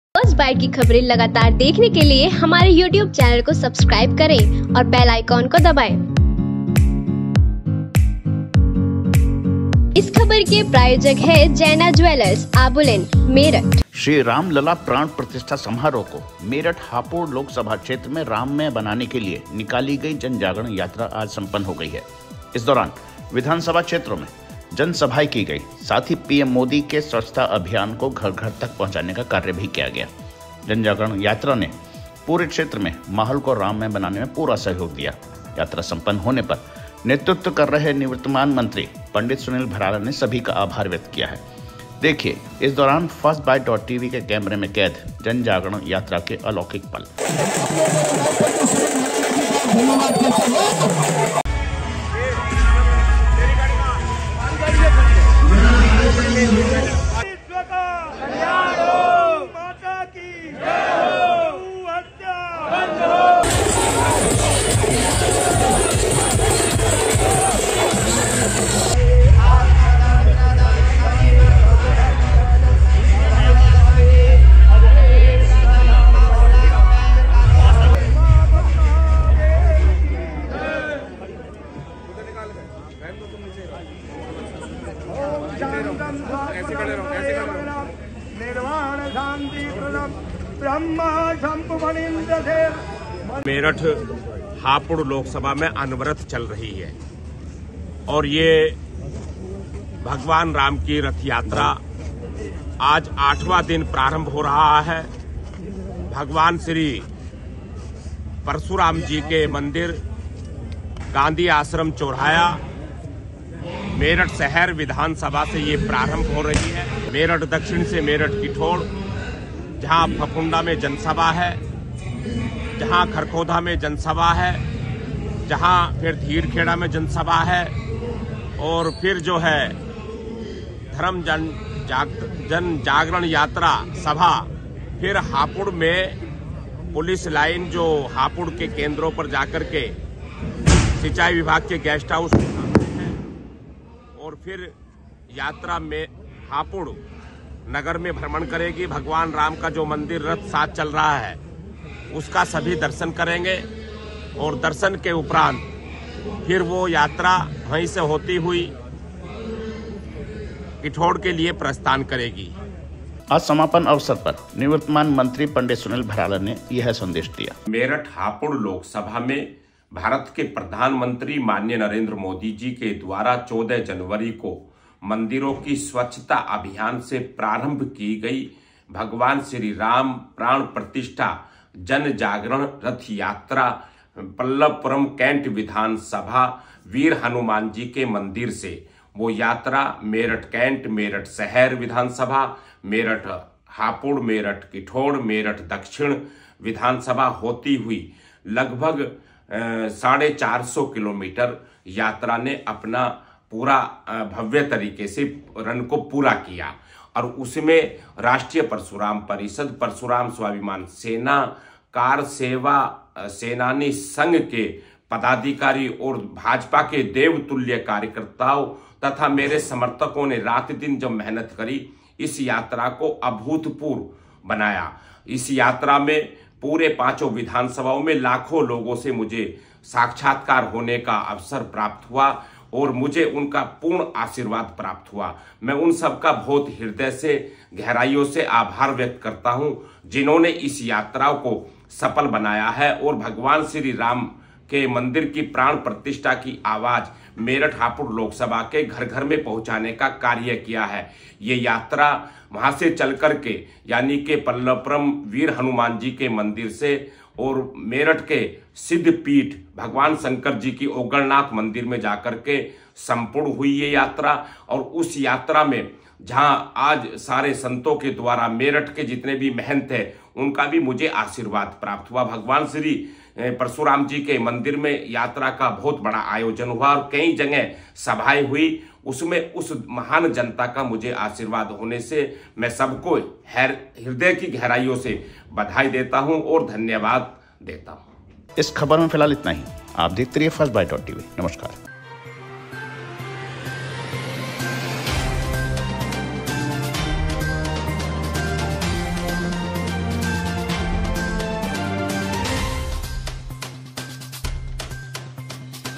राममय माहौल बनाने को निकाली जनजागरण यात्रा संपन्न - First Byet Tv
14 जनवरी से शुरू हुई जनजागरण यात्रा
मेरठ हापुड़ लोकसभा क्षेत्र में माहौल बनाया राममय
जगह-जगह सभाएं, नागरिकों का किया  भव्य स्वागत
निवर्तमान मंत्री पं.सुनील भराला के नेतृत्व में निकली यात्रा
कार सेवकों को  भी किया गया सम्मानित
श्री राम लला प्राण प्रतिष्ठा समारोह को मेरठ हापुड़ लोकसभा क्षेत्र में राम मय बनाने के लिये निकाली गयी जन जागरण यात्रा संपन्न हो गई है।  इस दौरान विधानसभा क्षेत्रों में जनसभाएं की गई, साथ ही पीएम मोदी का स्वच्छता अभियान घर घर तक पहुंचने का कार्य किया गया। जनजागरण यात्रा ने पूरे क्षेत्र में माहौल को राममय बनाने में पूरा सहयोग दिया। यात्रा संपन्न होने पर नेतृत्व कर रहे निवर्तामान मंत्री पं सुनील भराला ने सभी का आभार व्यक्त किया है
The Jan Jagran Yatra organized to make the Shri Ram Lala Pran Pratishtha ceremony Ram Meerut in Meerut-Hapur Lok Sabha constituency has been completed. During this period, public meetings were held in the assembly constituencies, as well as PM Modi's cleanliness campaign was carried out to reach every household. Janajagran Yatra gave full support in making the atmosphere pleasant in the entire area. Upon completion of the yatra, outgoing minister Pt. Sunil Bharala, who was leading, expressed his gratitude to everyone.